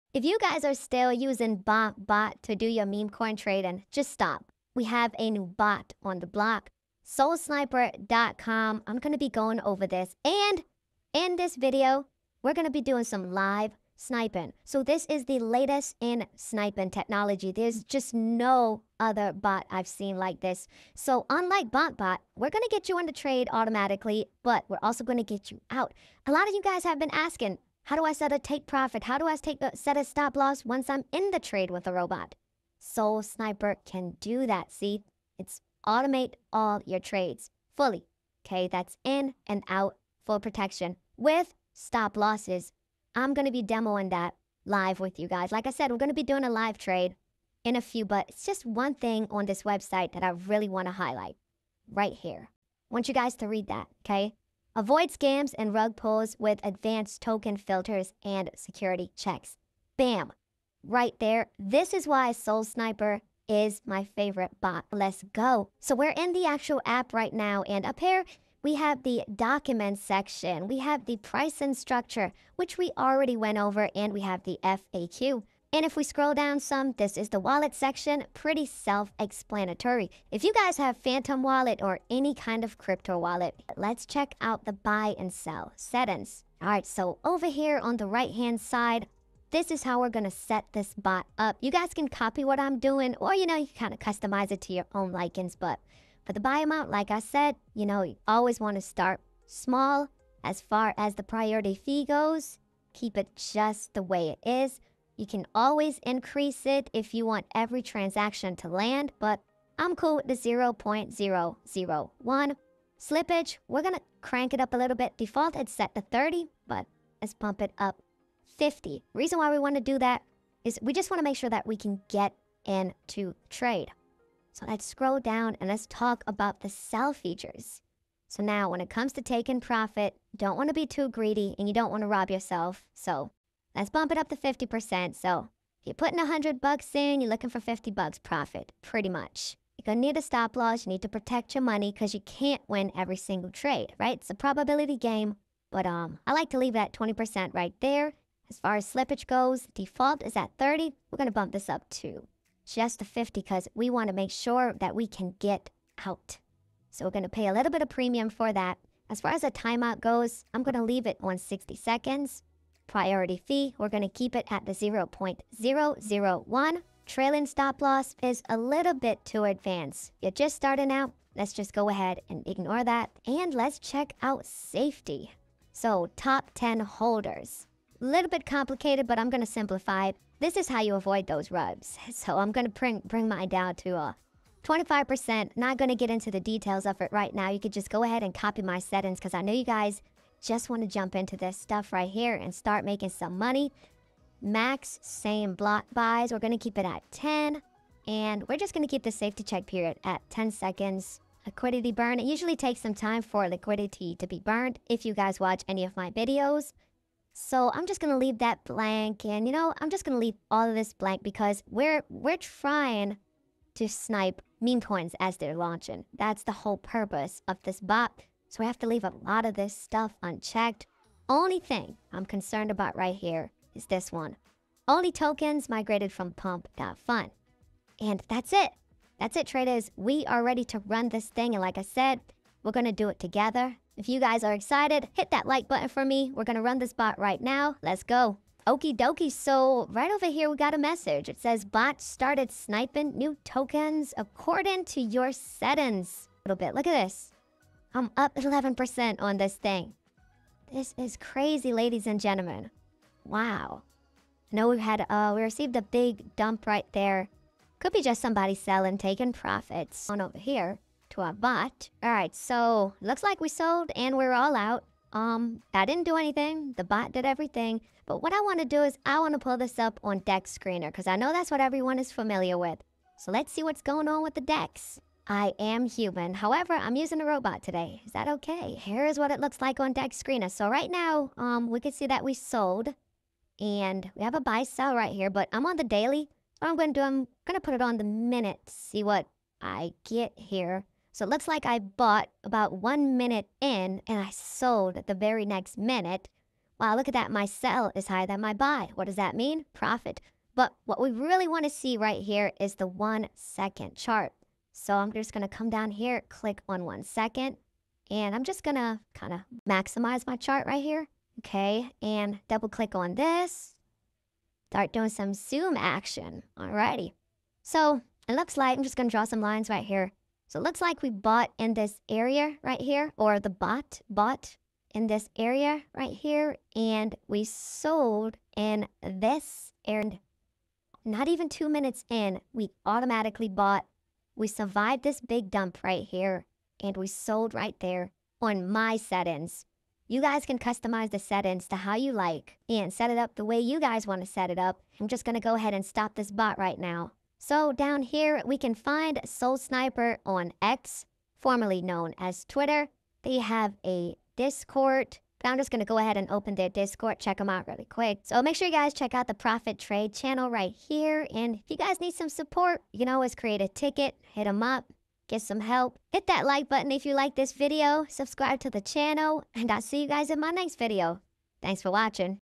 SOL SNIPER BOT! STEP BY STEP GUIDE IN 2025
Discover how to snipe Solana meme coins automatically using SOL Sniper! In this tutorial, I showcase how to set up and run a sniper bot for newly launched tokens on Raydium. 🔥

✔️ What You'll Learn:
 ✅ How to use SOL Sniper for automated trading
 ✅ Setting up buy/sell filters & risk management
 ✅ LIVE trade demonstration
 ✅ Avoiding rug pulls & maximizing profits

SOL Sniper is a powerful auto-trading bot that scans new Raydium listings in real time, placing trades based on customizable filters. Whether you're a beginner or an experienced trader, this bot helps you catch early pumps and avoid bad trades.